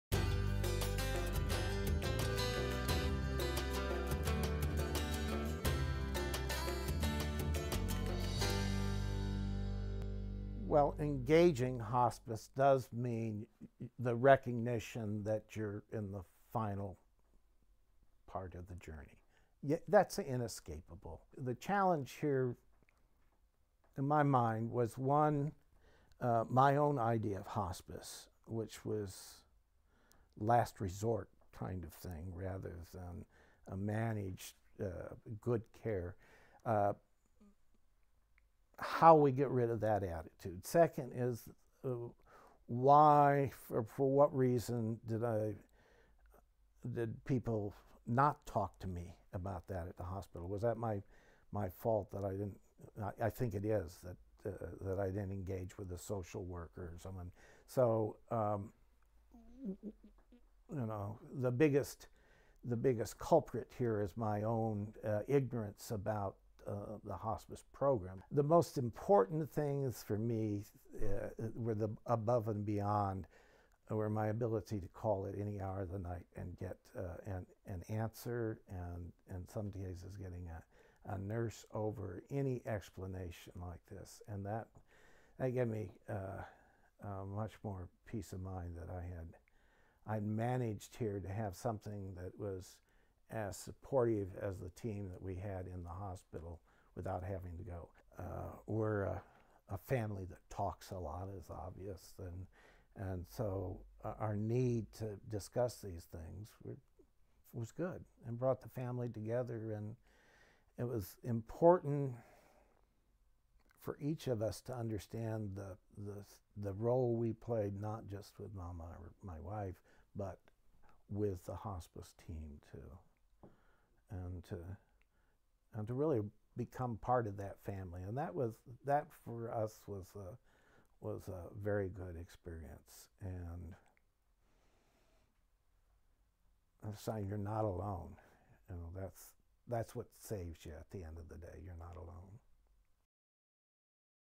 That's what saves you at the end of the day
Jon Wiant shares his ambivalence about engaging hospice for his wife Cay, and the ultimate benefits that their choice brought to his family.